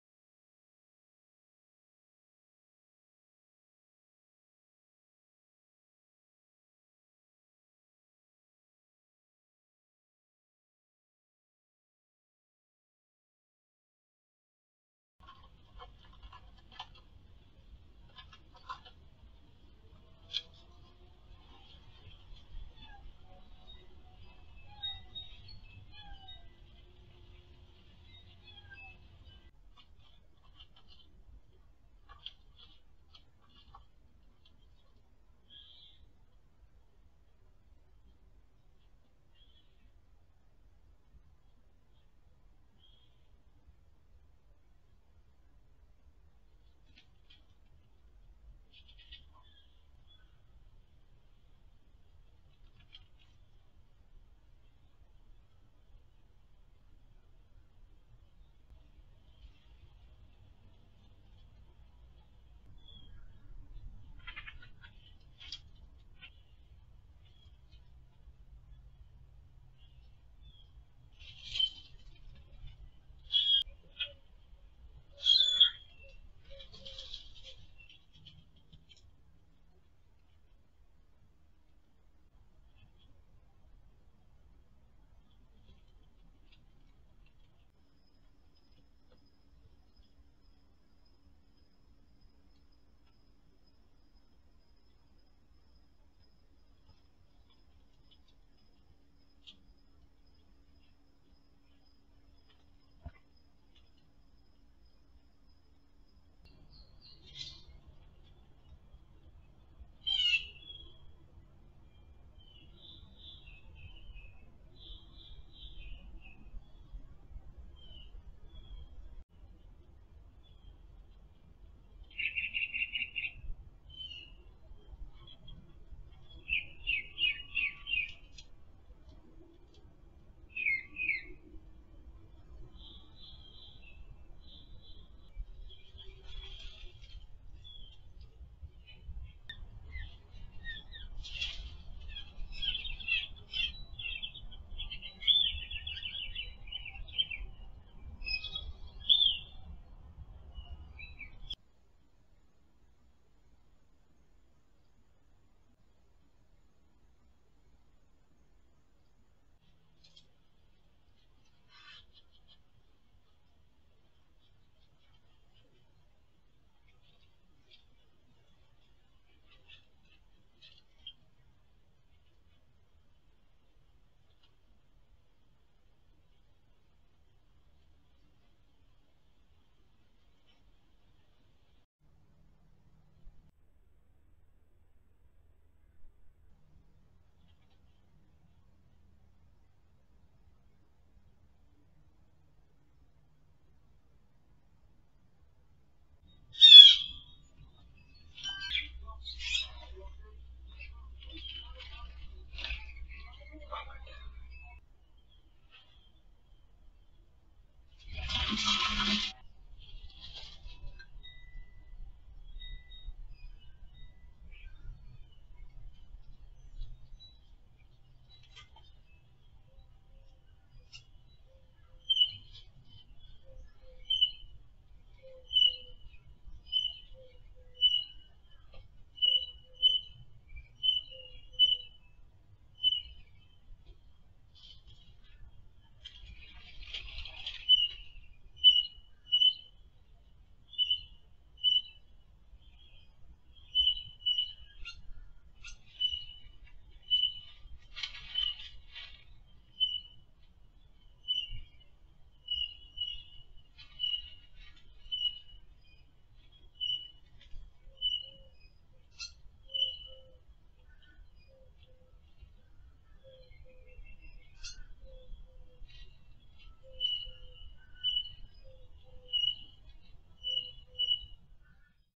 Security Camera Shenanigan's
A compilation of funny and strange security camera clips.